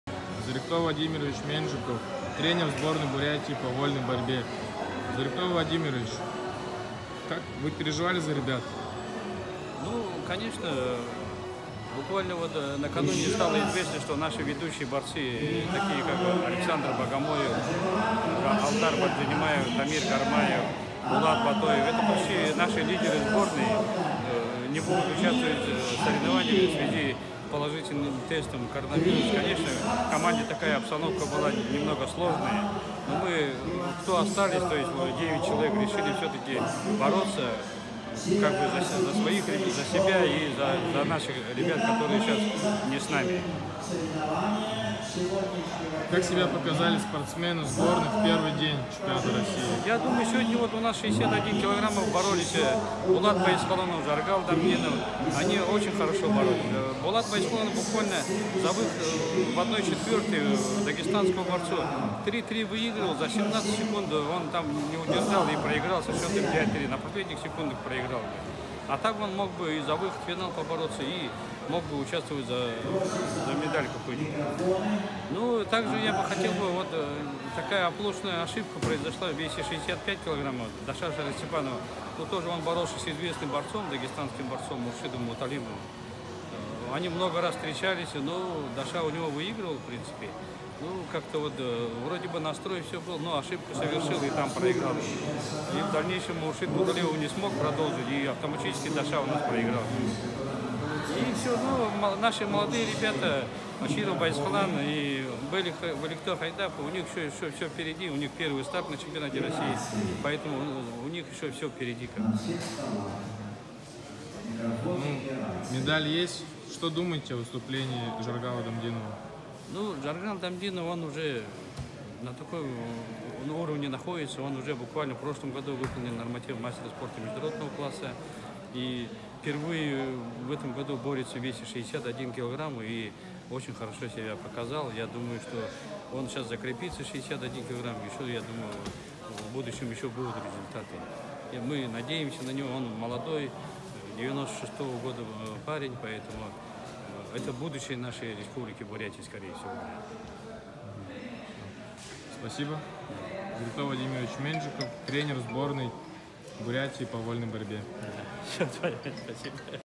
Зоригто Менжиков. Тренер сборной Бурятии по вольной борьбе.
Зоригто Менжиков. Тренер сборной Бурятии. О чемпионате России по вольной борьбе 2020. Наро-Фоминск.